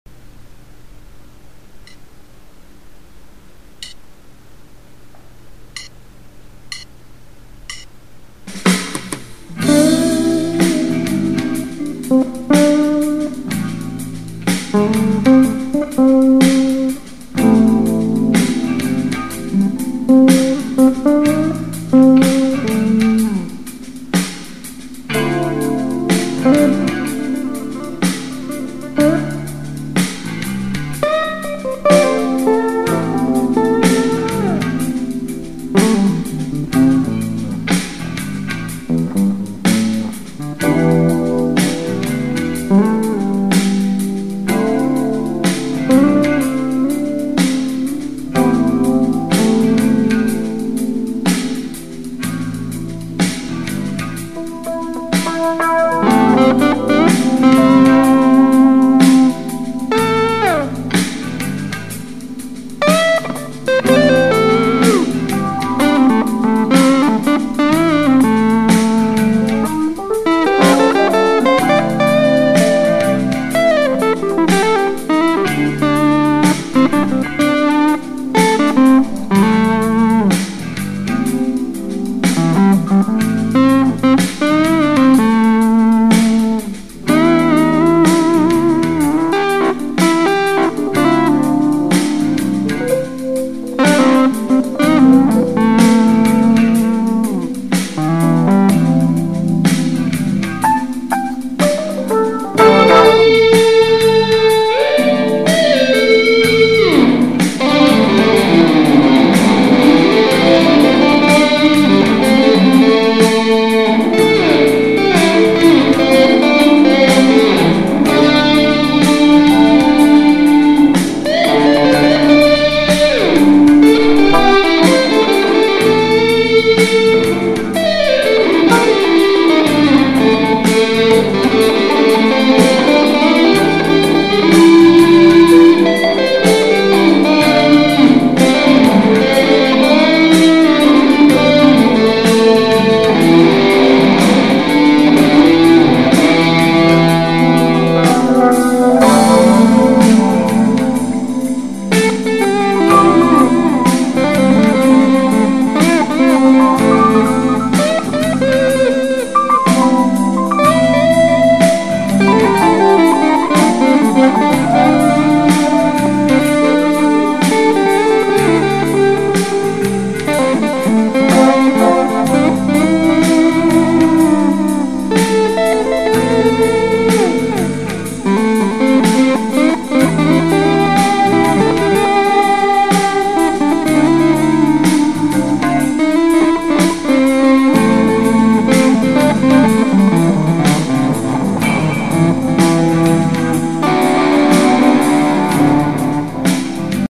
Les Paul Westone XL10 sound test GFS humbucker
Westone XL 10 Les Paul copy with GFS vintage 59' neck humbucker sound test